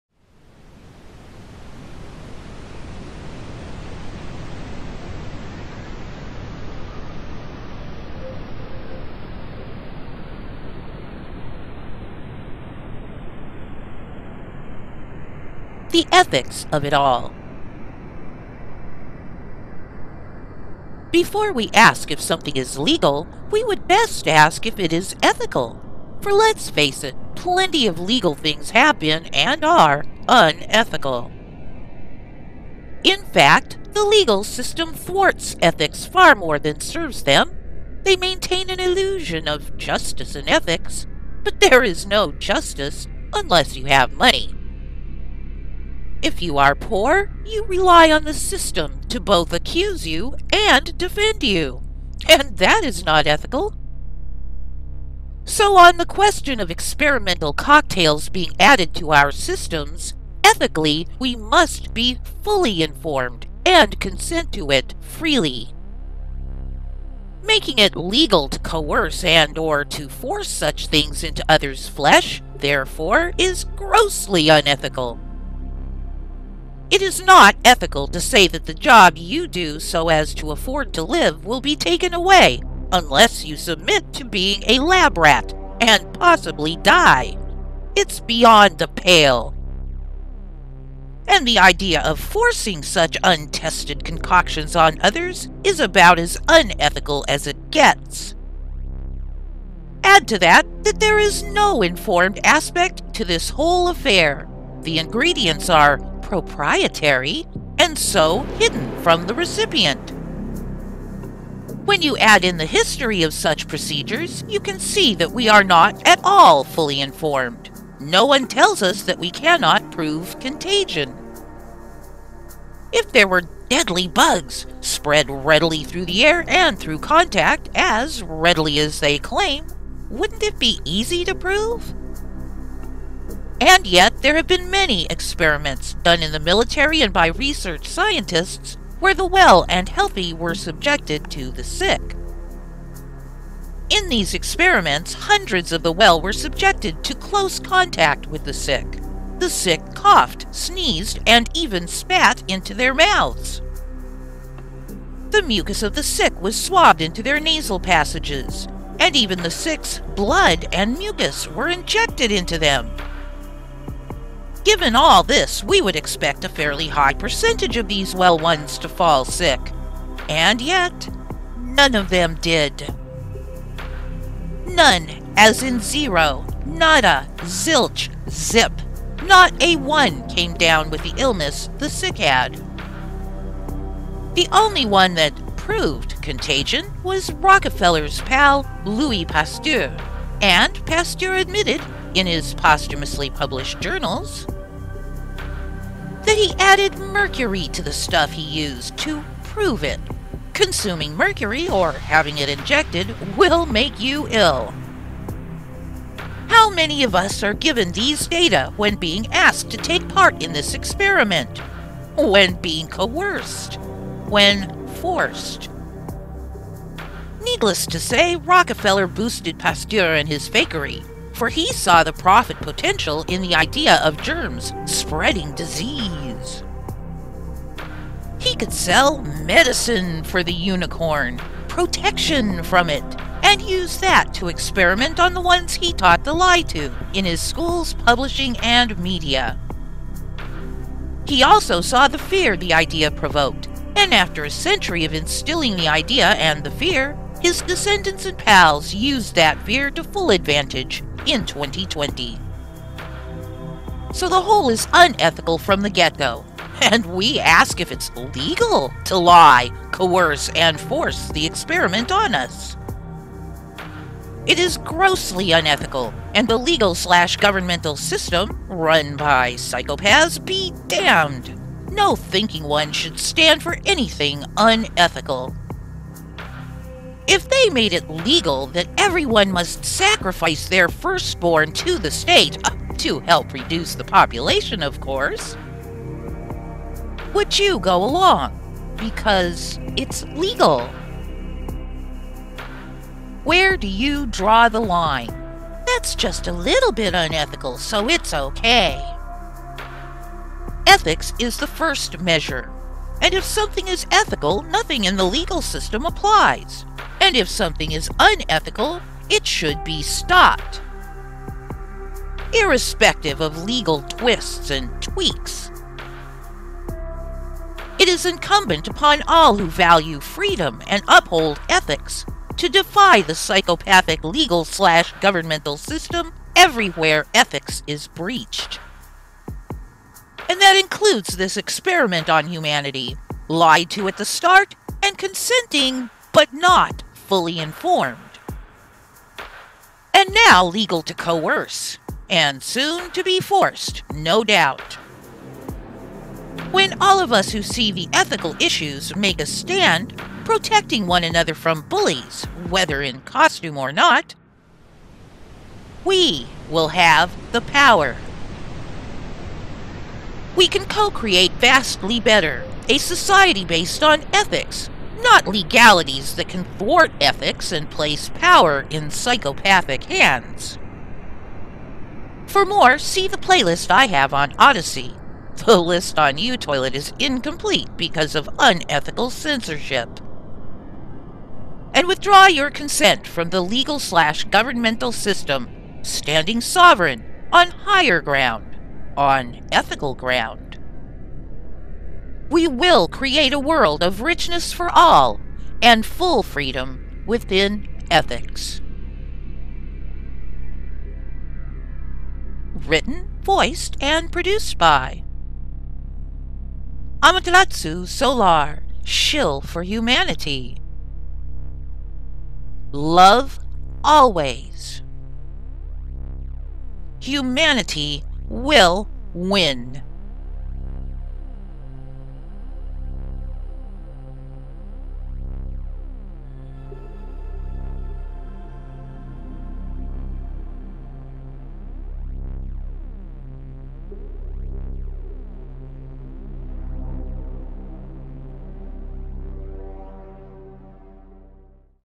The Ethics of It All
Will We just accept unEthical things?  Is there a better choice?  Watch and find out!

Quite the Opposite – A Short Story for Change – by Amaterasu Solar

My father taught Me never to believe anything.  He told Me to place probabilities and adjust them as new data come along, asking the question, "Does that explain what I see?," when evaluating data.  He was an aerospace engineer, and worked with T Townsend Brown (see My featured vid on YouTube).  From a very early age I was concerned that the way I was told things worked, in terms of government and social affairs, did not explain what I saw.  So the first few decades I worked to determine WHY this was.

And given I personally know that electrogravitics offers both gravity control and energy from the aether (the electromagnetic field that pervades the universe), and that it went into black projects, such efforts to hide and suppress would explain what I see completely.

Love always.